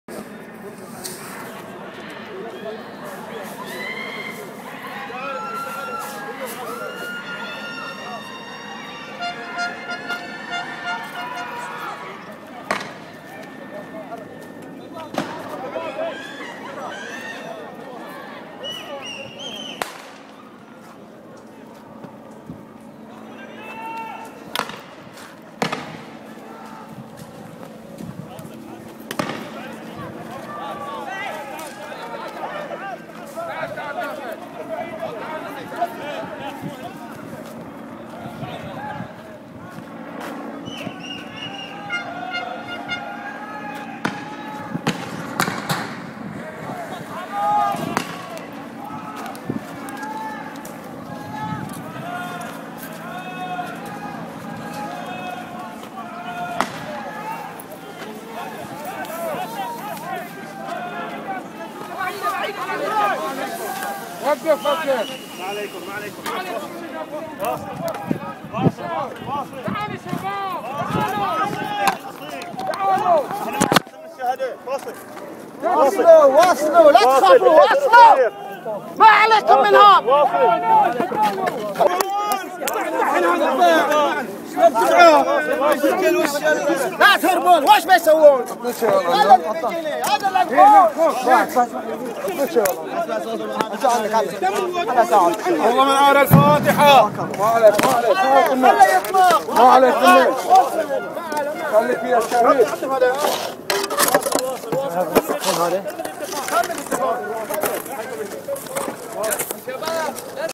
Bahrain: Suppressing Mourners During martyr Mansoor's Burial Feb18,2012
Bahrain: Police Mercenaries suppressing the mourners during the burial of Martyr Mansour Salman Hasan (85 y/o) in Sitra island on Feb 18, 2012